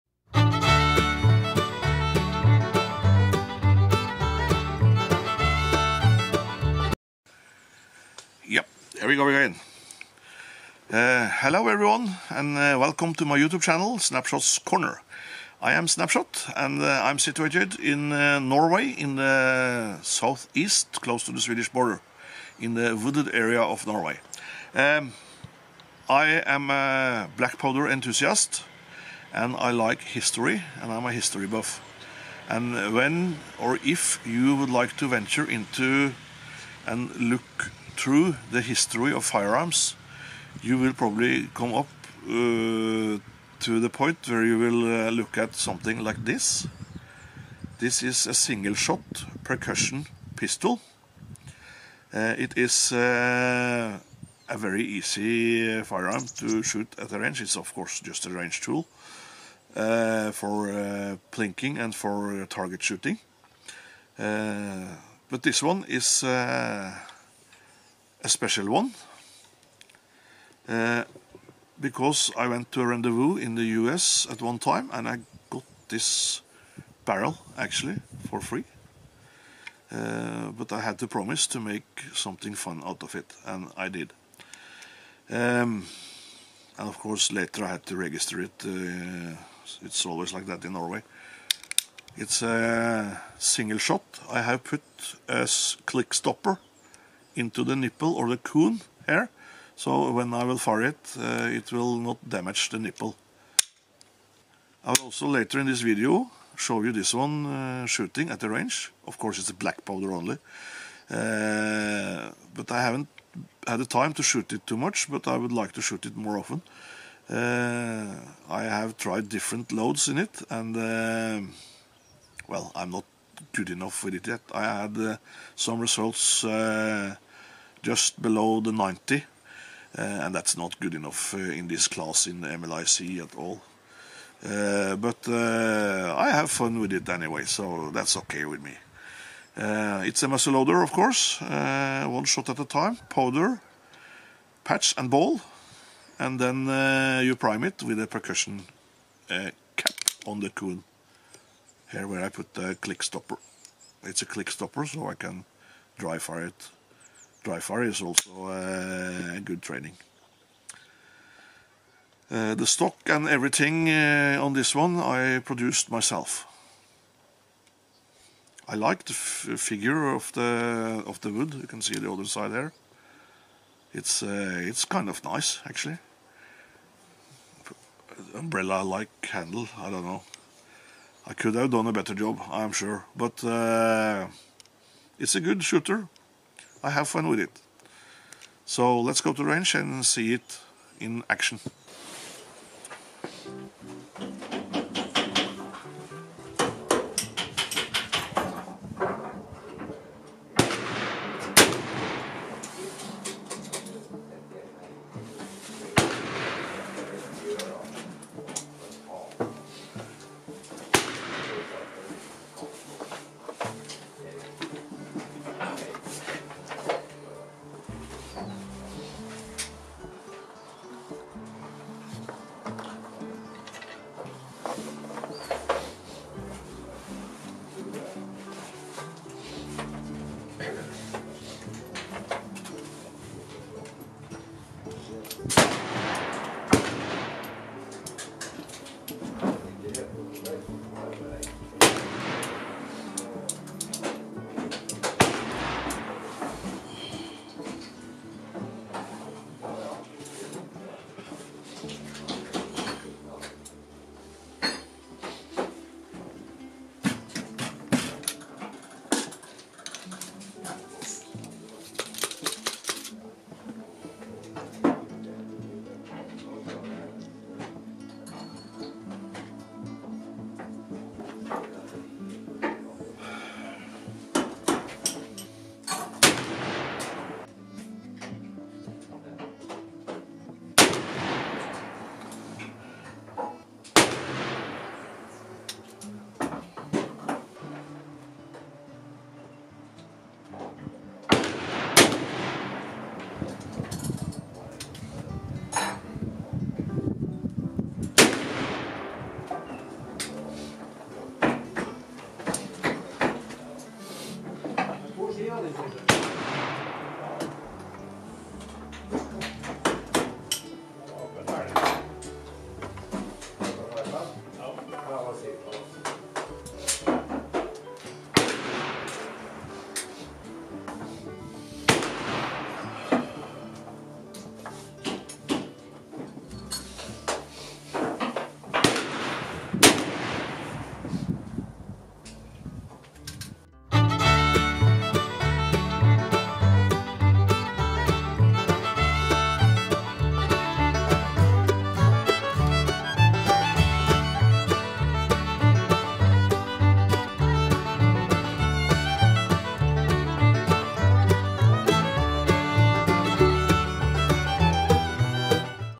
Percussion muzzle loading single shot pistol
A short talk on my percussion muzzleloading self made single shot pistol. We do go to a lokal competition to shoot it some.